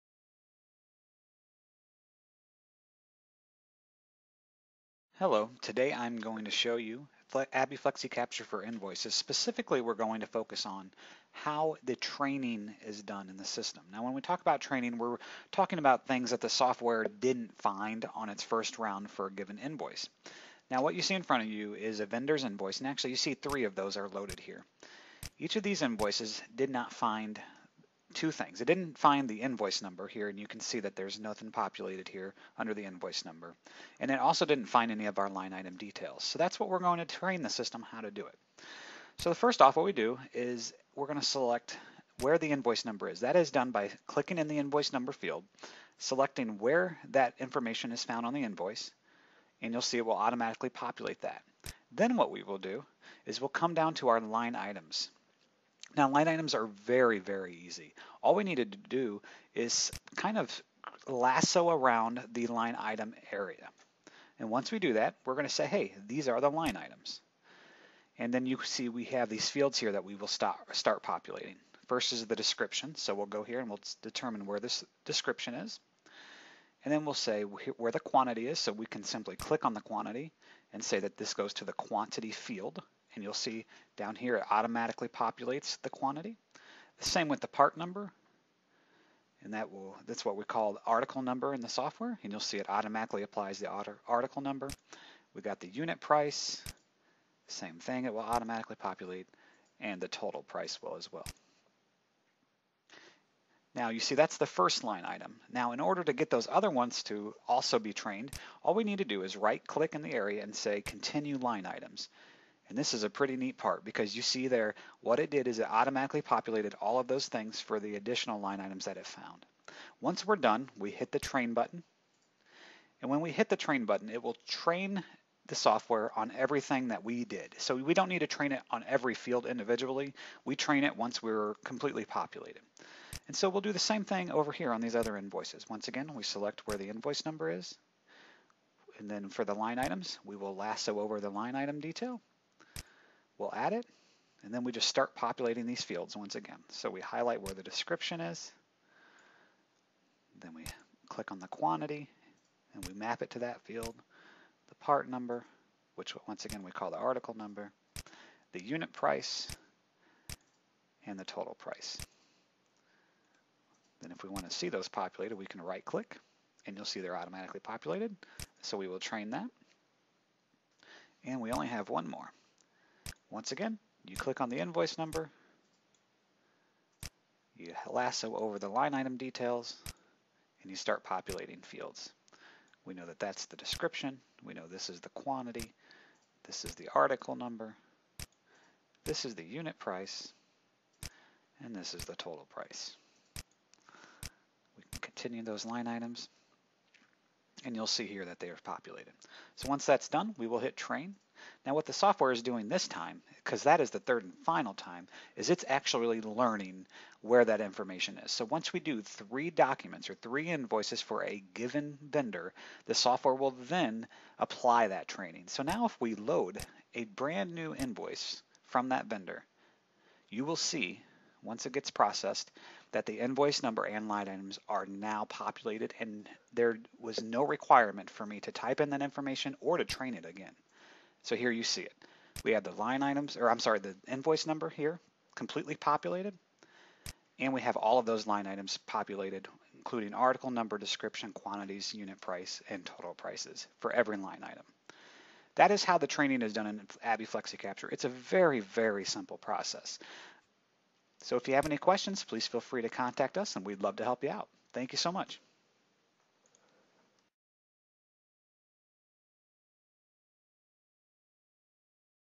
ABBYY FlexiCapture Operator/Accountant-based Training
Watch how Operator/Accountant-based Training is done with ABBYY FlexiCapture for Invoices! It doesn't get any easier than this!
*NOTE* This video is for FlexiCapture 11. If you have FlexiCapture 12, ABBYY has changed the process for training the software. You can find our video for this newer process

To find out more about UFC, Inc and the ABBYY FlexiCapture software please visit our